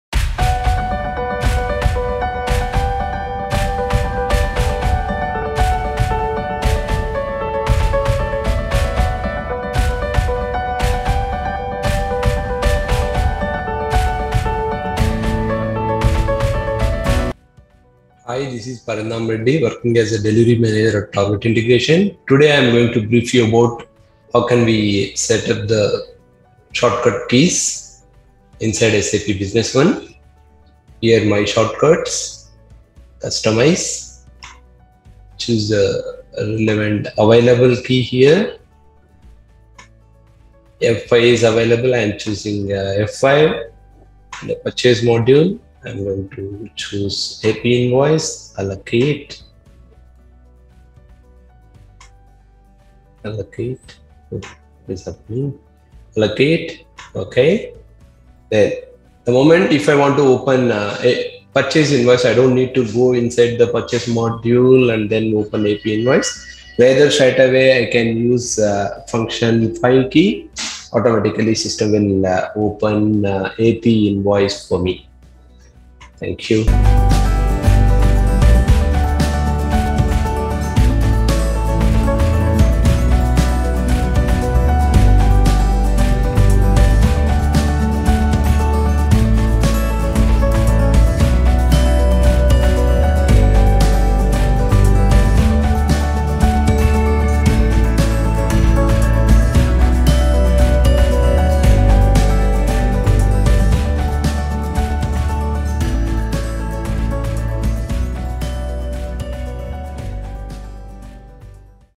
Hidden Features of SAP Business One | Additional Short Cut Key
Short cut keys play a good role in the everyday use of SAP Business One. After all the standard shortcut keys of SAP Business One, you can assign a particular function to a function key from time to time.

In this video, Target Integration’s SAP Business One Consultant, Parandhama Reddy demonstrates one of many “special” features of SAP Business One.

Need more tips? Follow the playlist: [play list of SAP Business One]